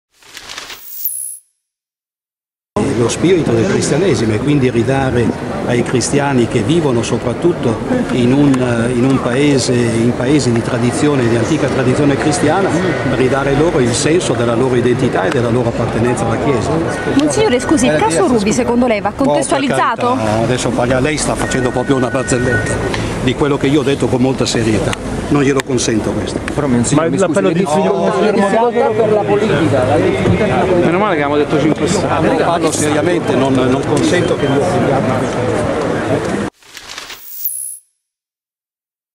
Caso Ruby, Mons.Fisichella: "Contestualizzarlo? Ma non mi offenda" e scappa via
Rino Fisichella, Arcivescovo e Vescovo già ausiliario di Roma, fu pronto ad assolvere il premier quando questi raccontò una barzelletta in cui vi era una bestemmia. Dichiarò allora il prelato: "E' necessario contestualizzare". Così oggi quando una giornalista della trasmissione Agorà (Rai3) gli ha chiesto: "Monsignor Fisichella, contestualizza il caso Ruby?" il monsignore è saltato su tutte le furie: "Ma per carità: lei sta facendo una barzelletta di quello che io ho detto, questo non lo consento - e ancora - qui io parlo seriamente e non consento che mi offendiate in questo modo".
Riprese di David Perluigi